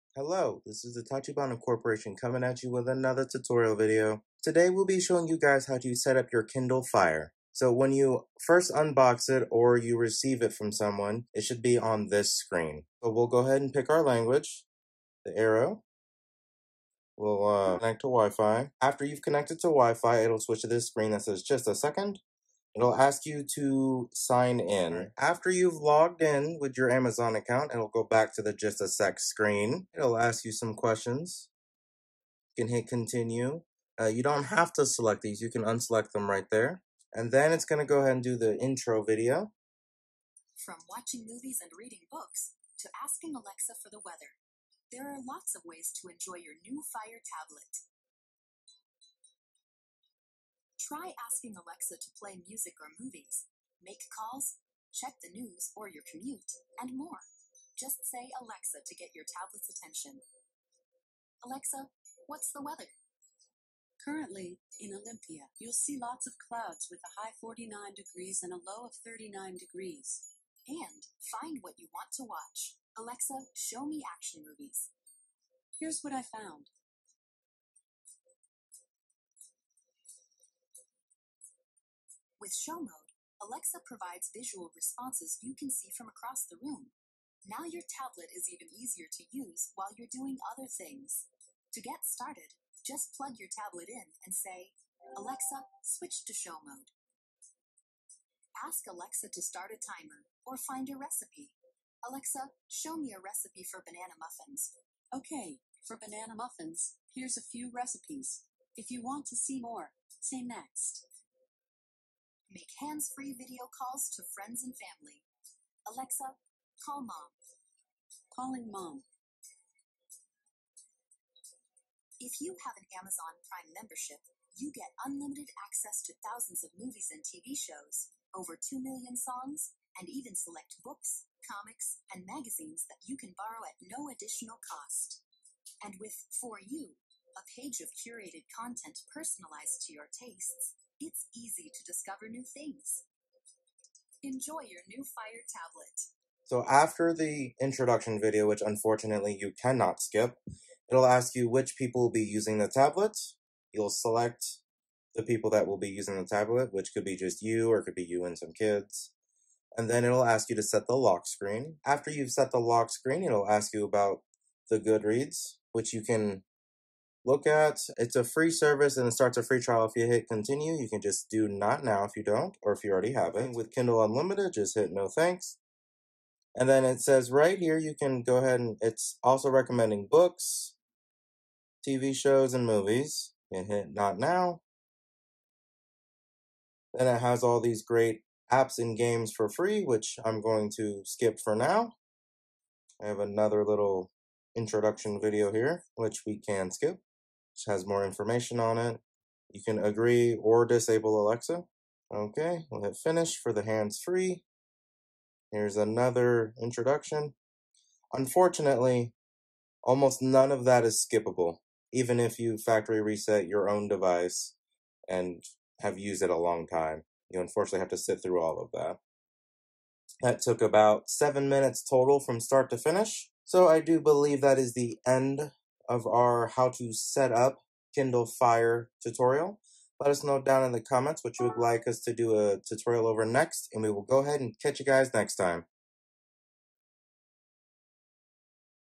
Tutorial: Amazon Fire Tablet Setup
This tutorial is to show you guys how to setup a Kindle Fire (Amazon Fire) device which covers any Kindle (Fire) that runs FireOS. We will be going through step by step the simple automated process and show you gut what will be needed to get it fully setup. This tutorial can be helpful if you inherited a Kindle, got one used, or got one new from Amazon either way we hope that you enjoy your new Amazon Fire device.

-- Gear Used --
Studio Lights
Apple iPhone 12 Pro Max
Anti-Static Mat

-- Credits --
Presenter: Tyrel Tachibana
Editor: Raven Tachibana
Thumbnail: Raven Tachibana